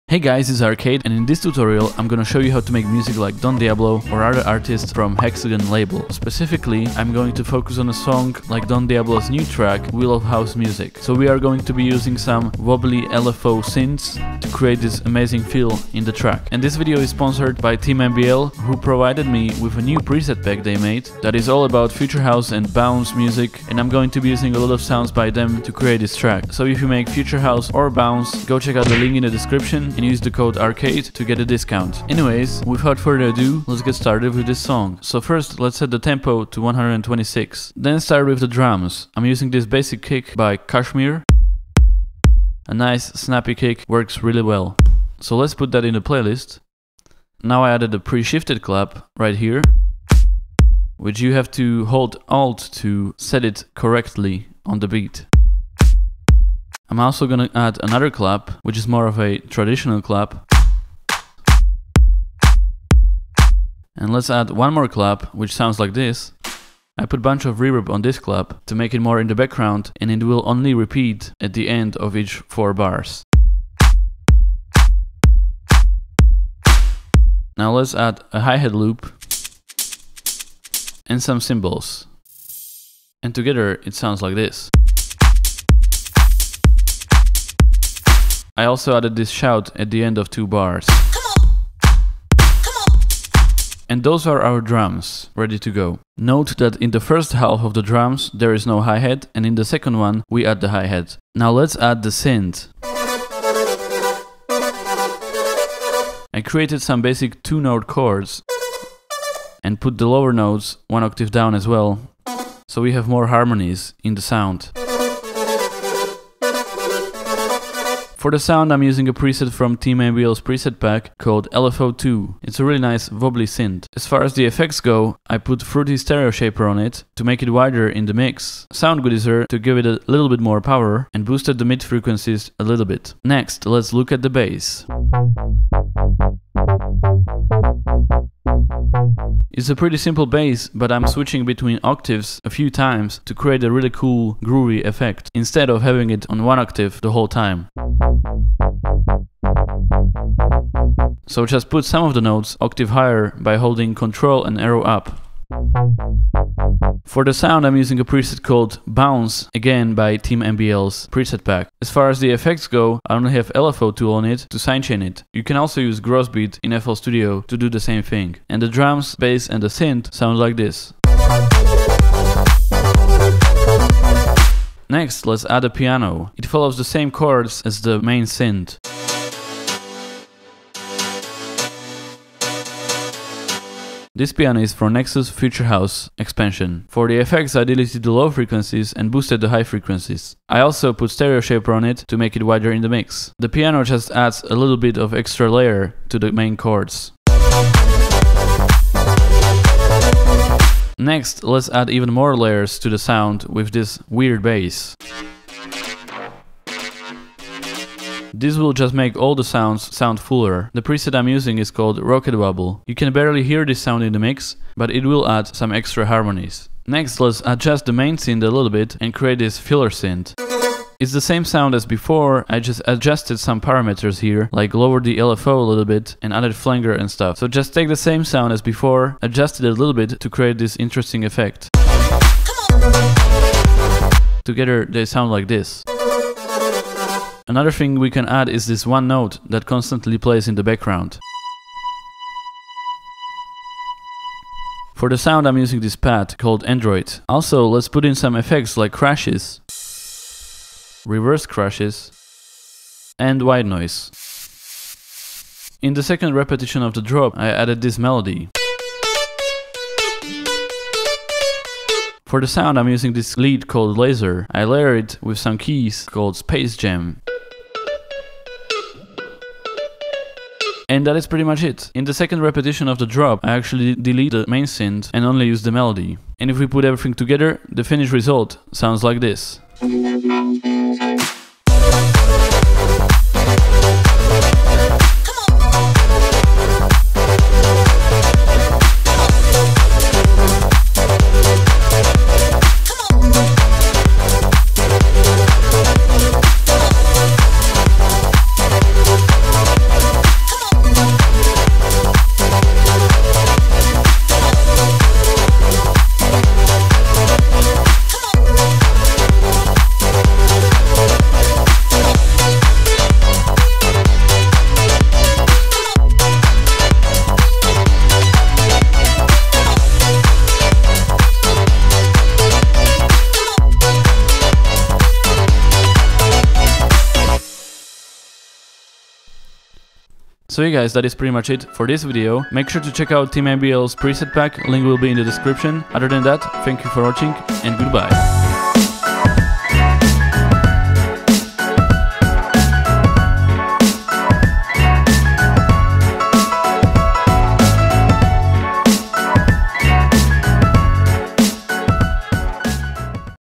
HOW TO DON DIABLO IN 5 MINUTES | FL Studio Tutorial
In this tutorial I am going to show you how to make music like don diablo or his hexagon label in under 5 minutes. I did a similar video before but this time we will be focusing on a different song and I am going to do it in a faster style.